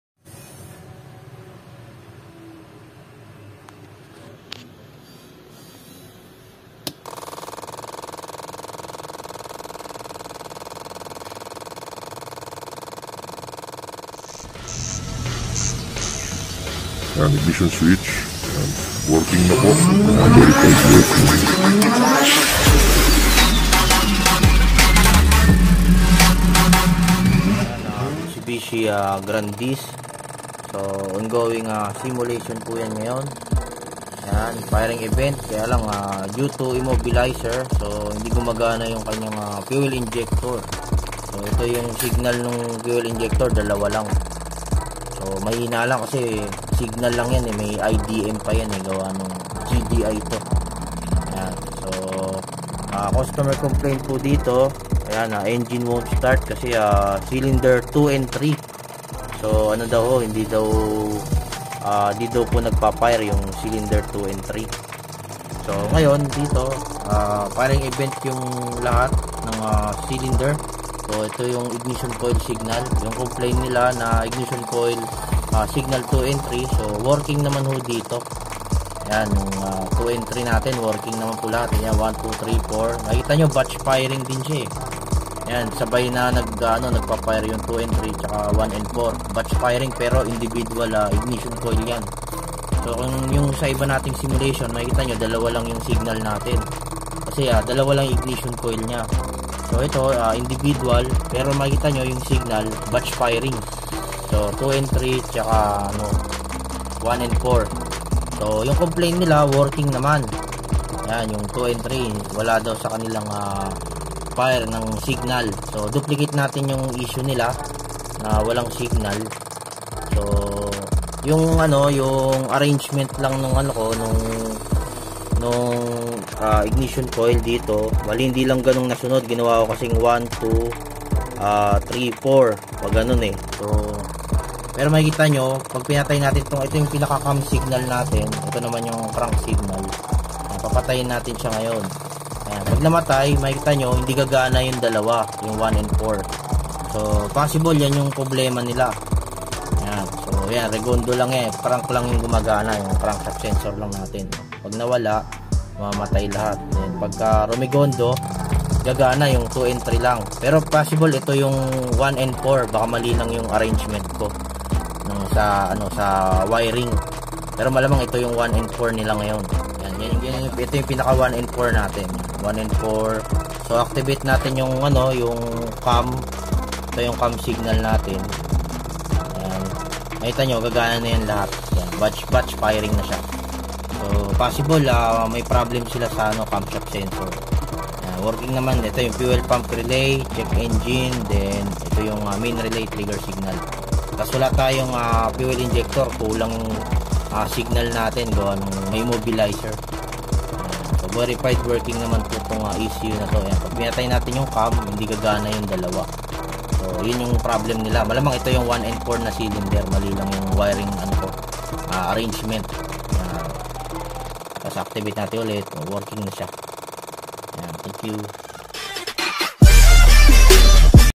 MITSUBISHI GRANDIS ECU SIMULATION
Customer complaints engine won't start due to no ignition coil output signal cylinder 2 & 3
(Suspected defective)

Simulated:

✅ 5v reference voltage
✅ Mainrelay trigger signal
✅ Fuel pump relay trigger signal
✅ Individual ignition coil signal
❌ Individual fuel injector signal due to immobilizer

Verified working 😃
Duplicated issue when no camshaft sensor signal
No ignition coil signal 2&3